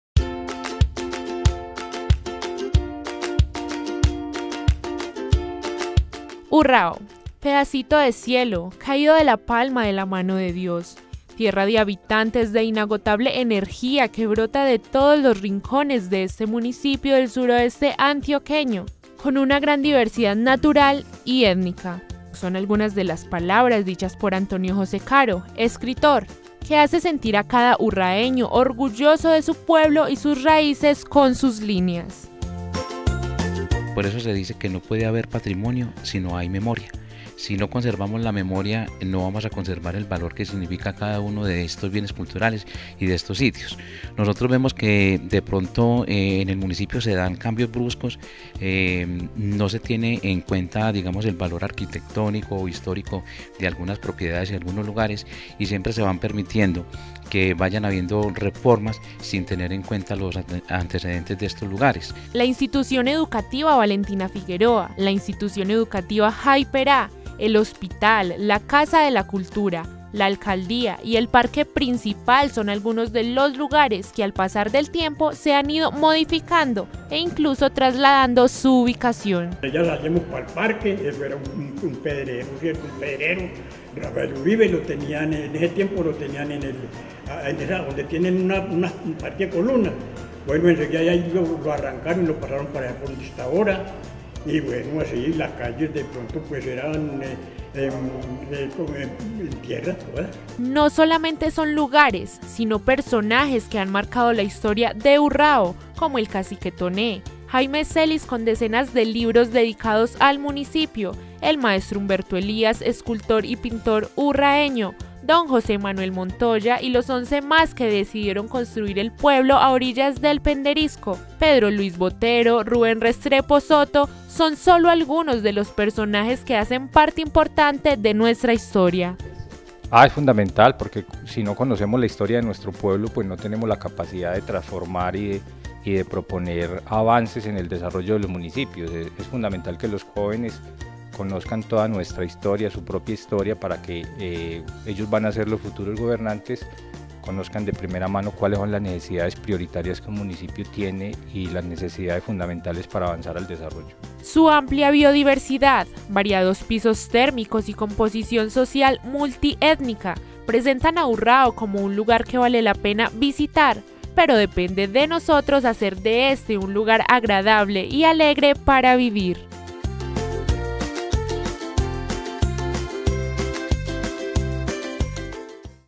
Urrao un Paraiso lleno de Historias
En nuestro municipio han habido pintores, escultores, escritores, músicos, y muchos otros personajes destacables, así como lugares y momentos que han marcado la historia de nuestro pueblo